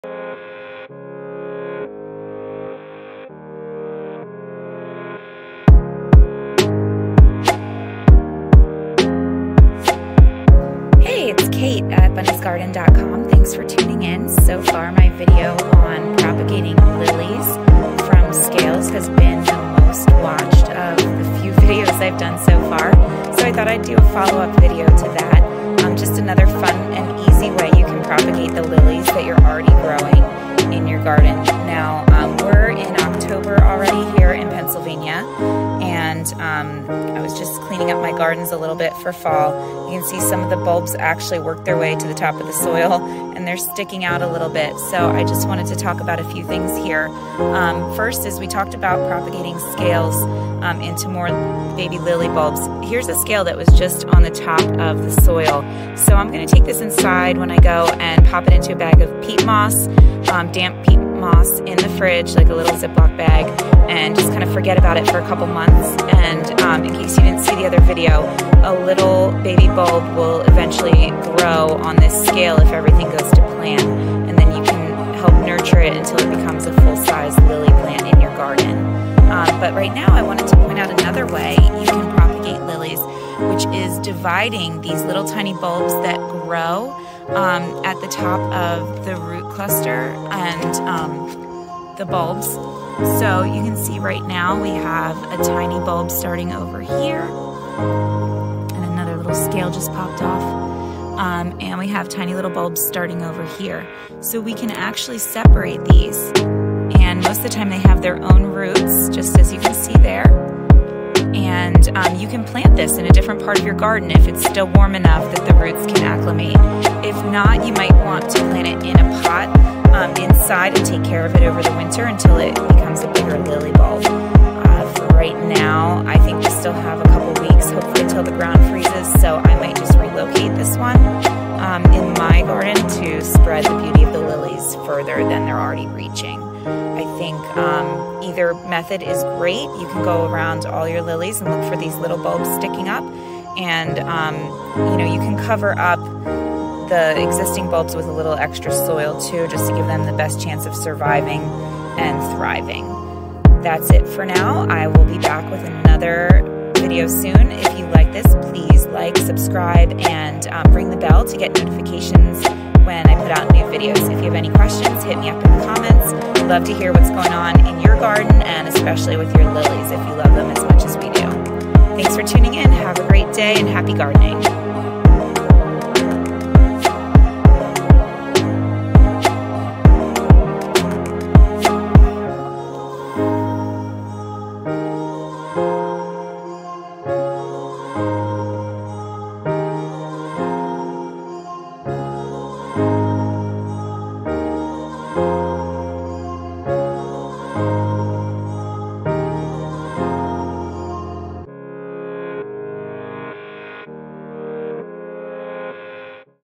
Propagating Lilies By Division - Separating Baby Bulbs You Can Replant!
Get more beautiful lilies in your garden by separating those you already have!

More lily propagation back by popular demand! Our video on propagating lilies from scales got the most attention of all our videos so far, so I made this one special to share another way you can get more lilies for FREE in your garden!

Watch and learn to Propagate Lilies from existing bulb clusters!!

Gently divide lilies by removing baby bulbs from the cluster around the mother lily. Replant those tiny lily bulbs elsewhere to spread the beauty of lilies further throughout your garden.

Do you have questions or stories to share? Hit us up in the comments!

Happy Gardening!